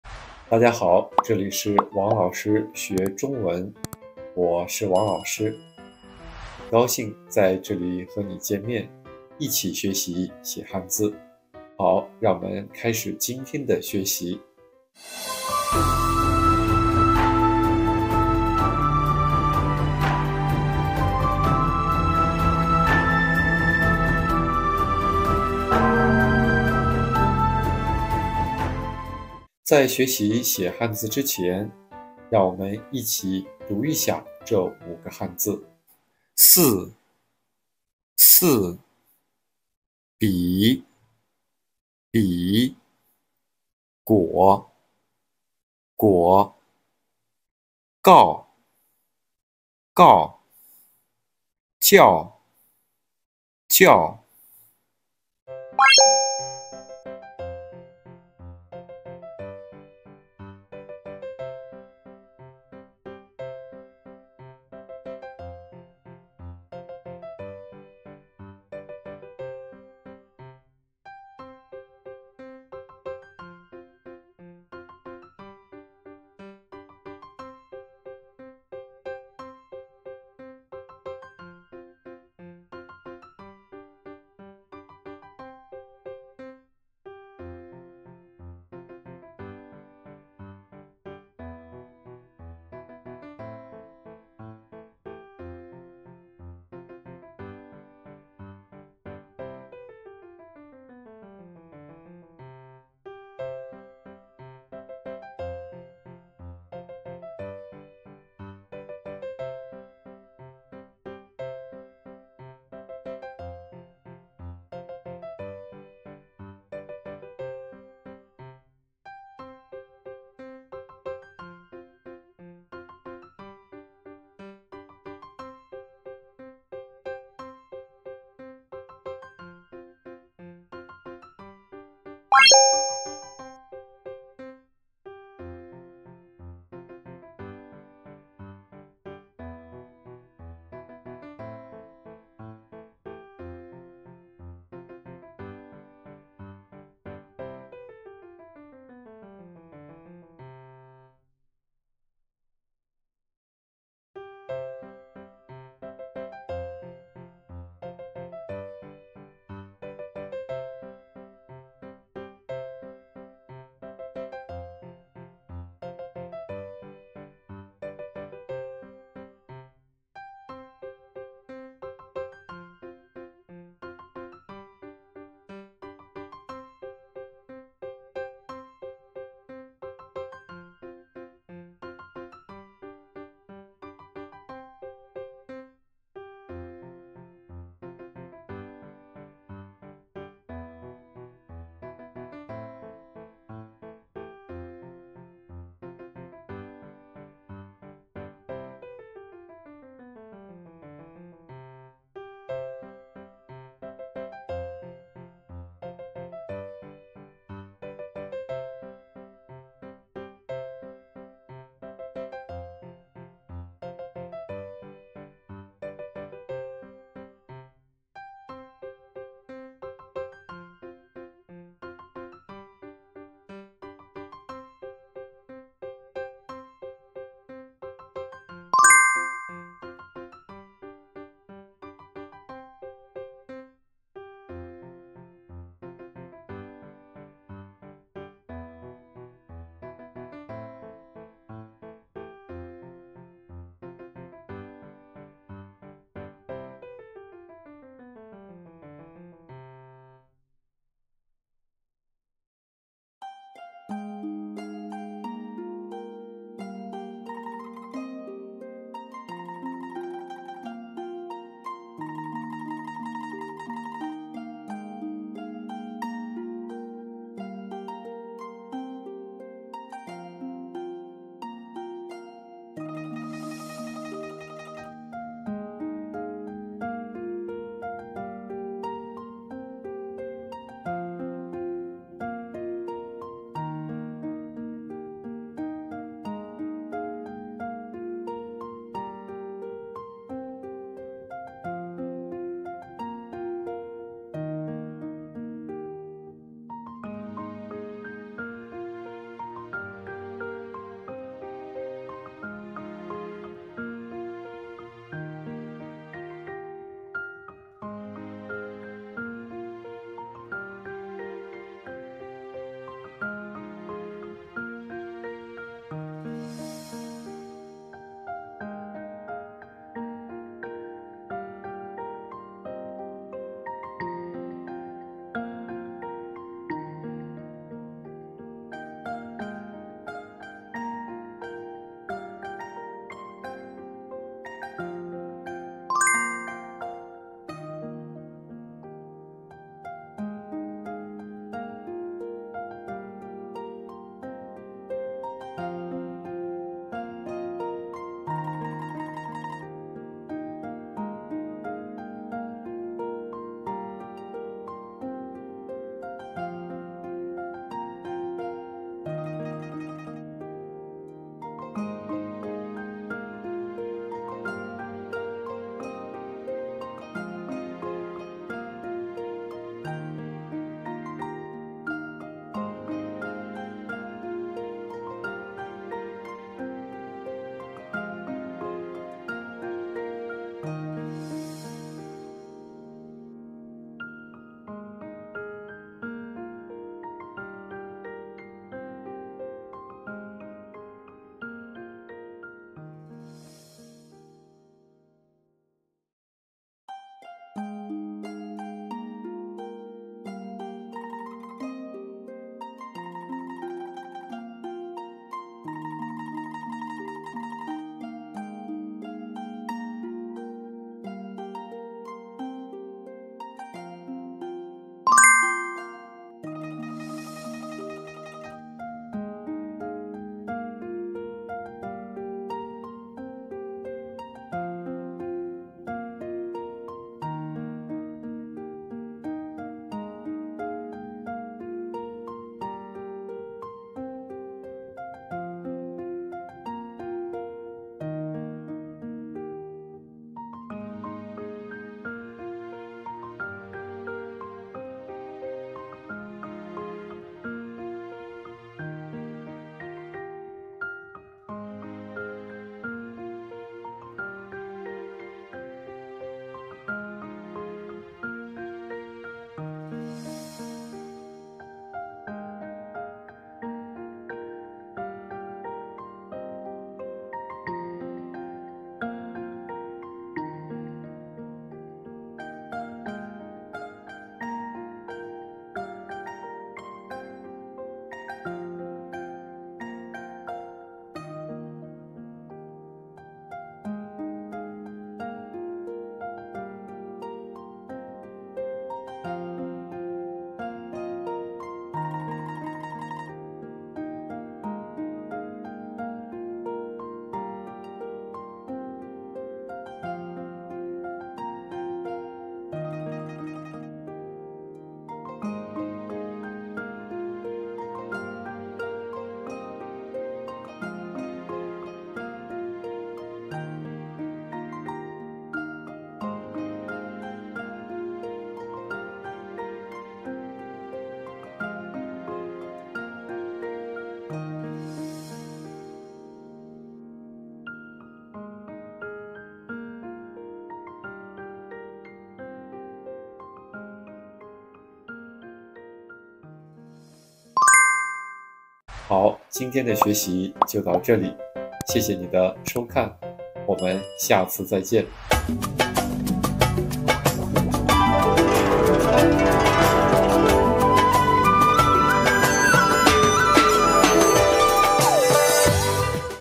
每日写汉字“四、笔、果、告、教”write Chinese characters every day
写汉字不再是困难的事情！每天学习五个汉字的书写方法，熟悉汉字书写习惯，书写时请注意正确的笔画顺序，每个汉字的结构。跟我一起写汉字吧！Writing kanji is no longer difficult! Learn how to write five Chinese characters every day, get familiar with the Chinese writing habits, and pay attention to the correct order of strokes and the structure of each Chinese character when writing. Write Chinese characters with me!